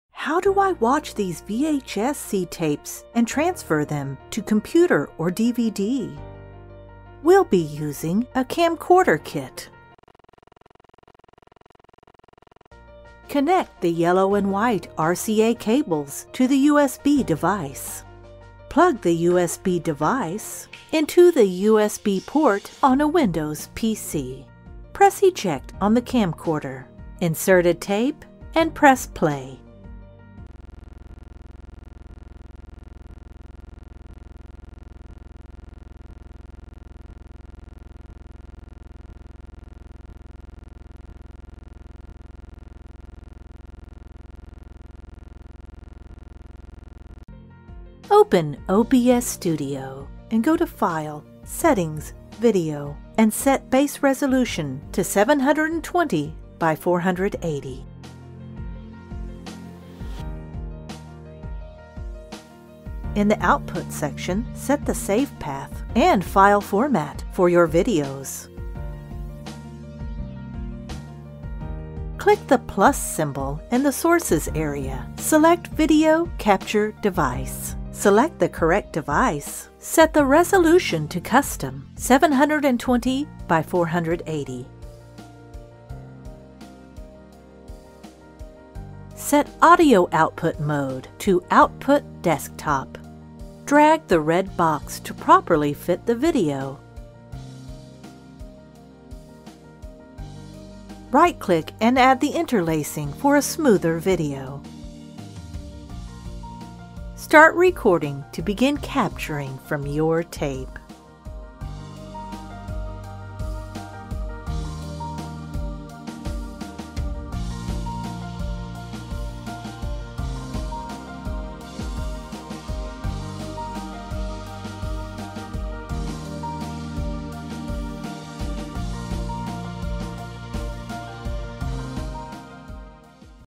How do I playback VHS-C tapes and transfer to USB or DVD?
Camcorder kit

USB adapter